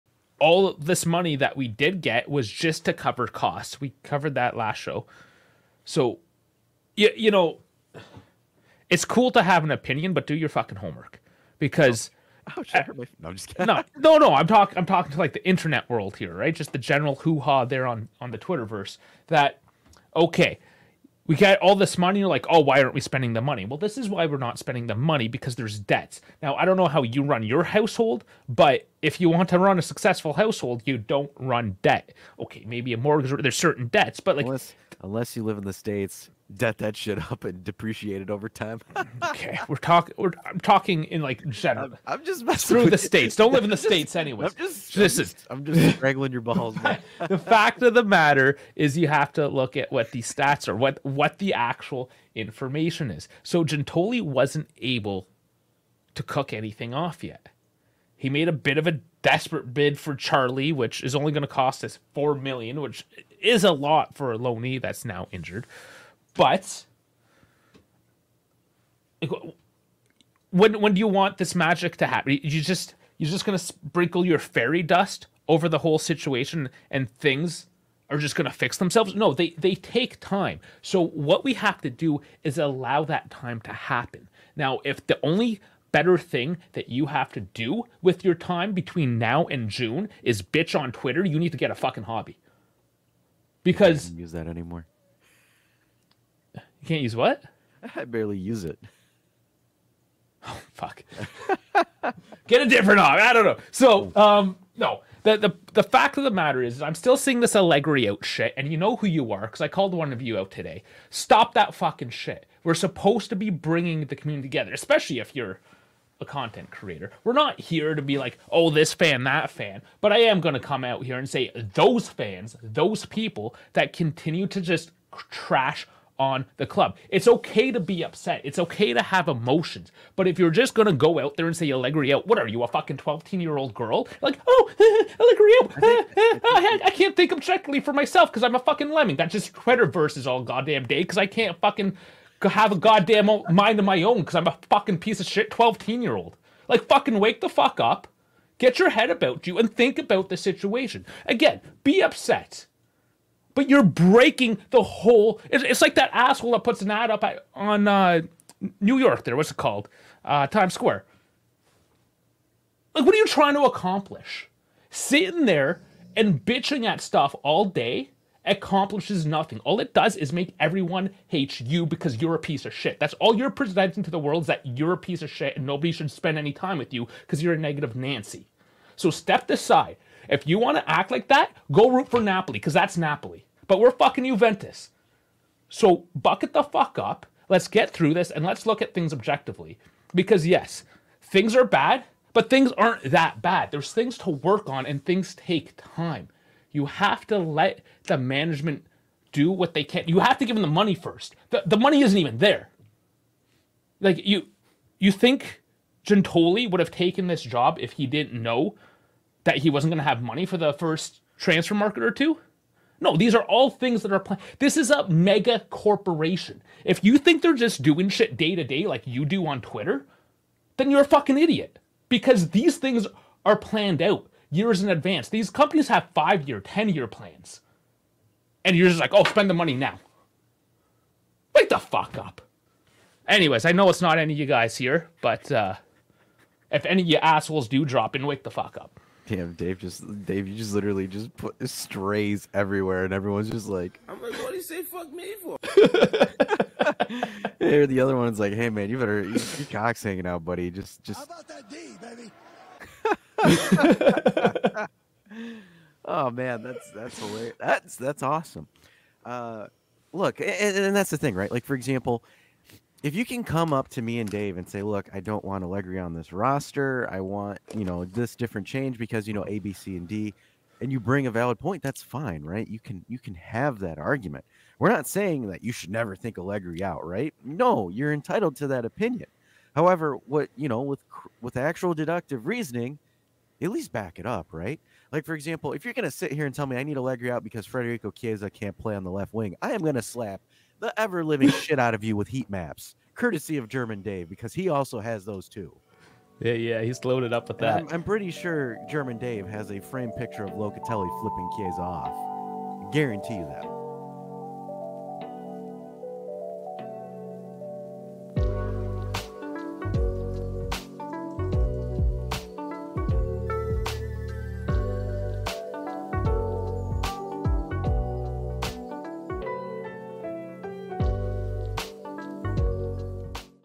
J Twitter - Dave goes on a RANT!
Dave loses his mind and goes on a rant.  It's J Twitter this time.  This has to be good.